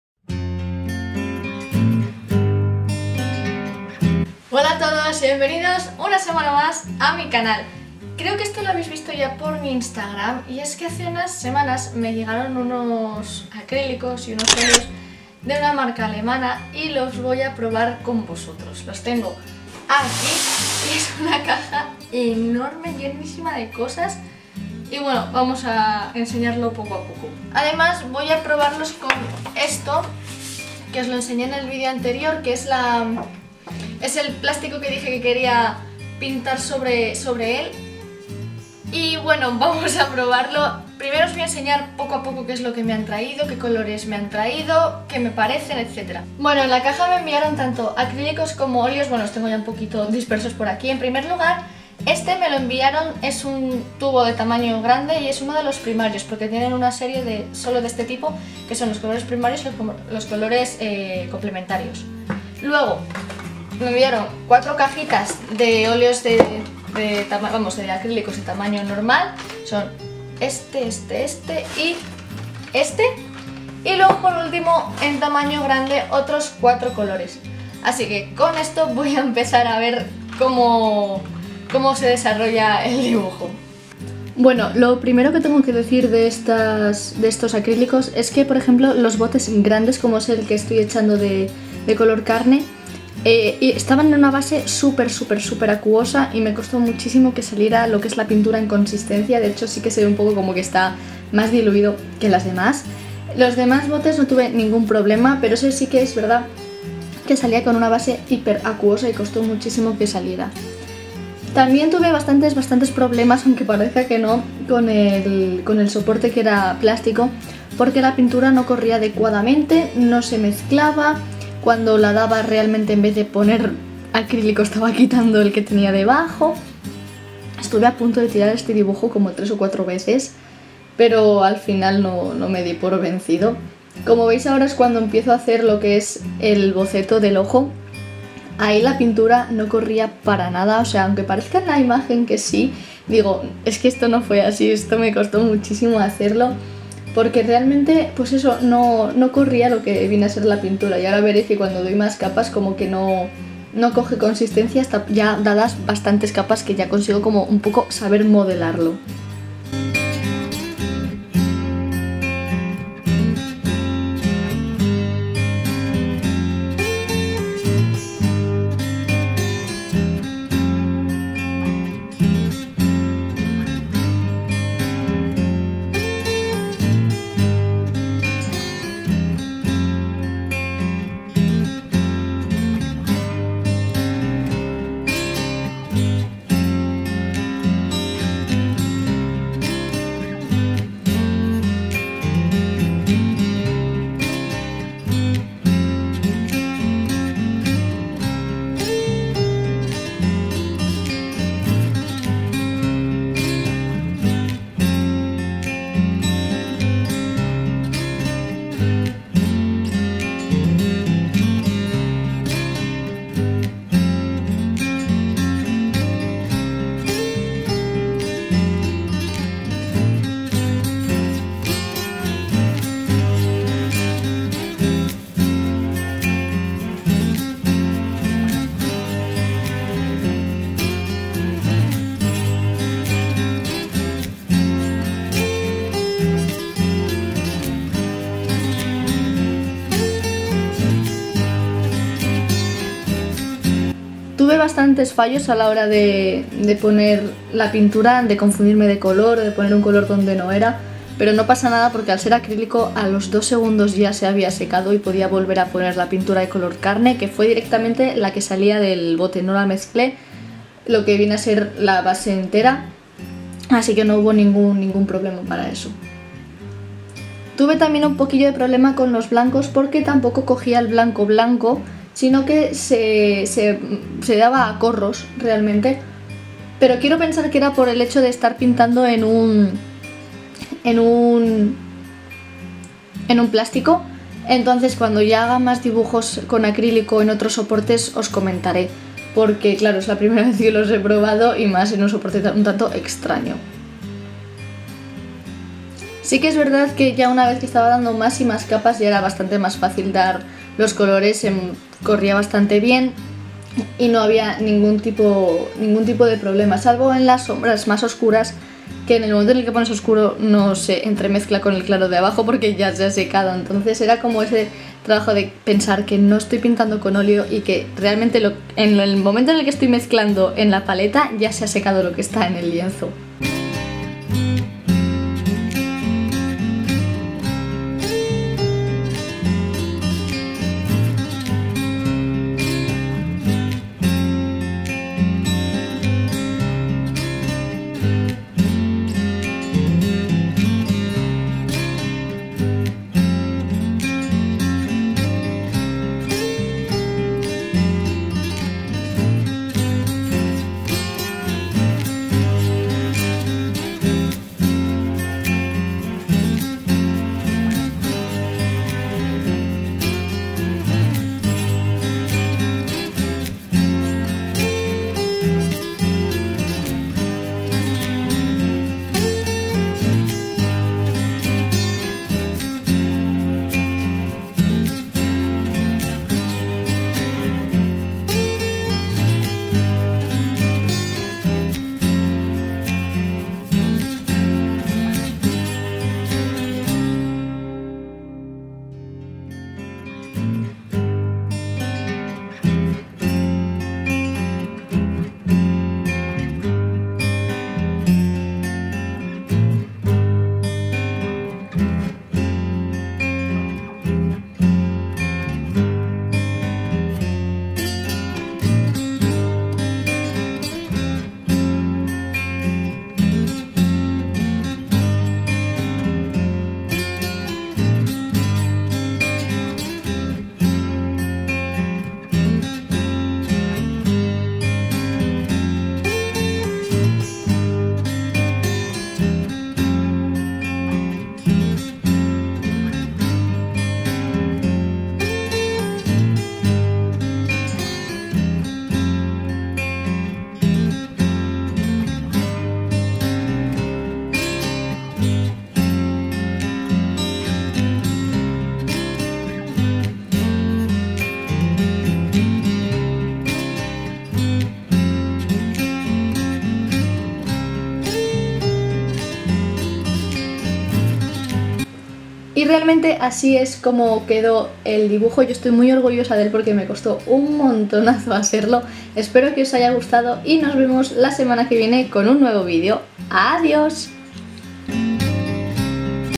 SPEED PAINTING PLASTIC EYE | Celia Gallego
¡Ábremeeeeee!
¡Hola hola! Ya estamos a domingo de nuevo y esta vez os traigo un speed painting sobre un soporte de plástico, la verdad me costó bastante este dibujo, pero espero que os guste tanto como me gustó a mí hacerlo, ¡y grabarlo!